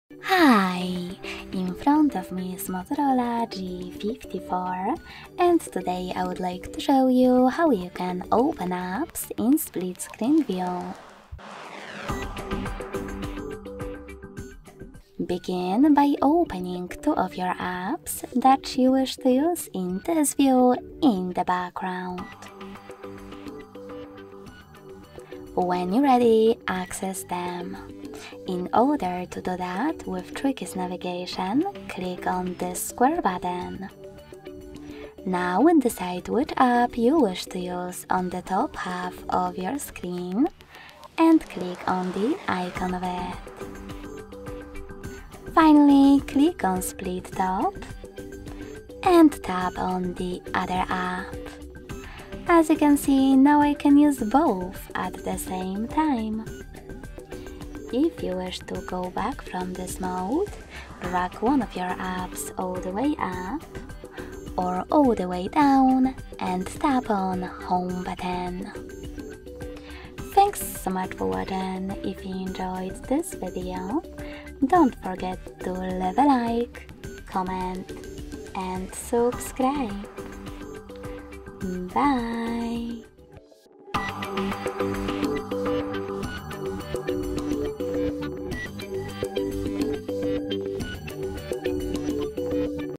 How To Enter Split Screen In Motorola G54
Enhance multitasking on your Motorola G54 by learning how to enter split-screen mode. In this tutorial, we guide you through the steps to use the split-screen feature, allowing you to run two apps simultaneously and boost your productivity. Watch now to discover how to make the most of split-screen functionality on your Motorola G54.

How do I enter split-screen mode on my Motorola G54?
Where can I find the split-screen option within the device interface?
Why would I want to use split-screen on the Motorola G54?
Can I choose different apps to run in split-screen mode?
What should I do if the option to enter split-screen is not visible or accessible?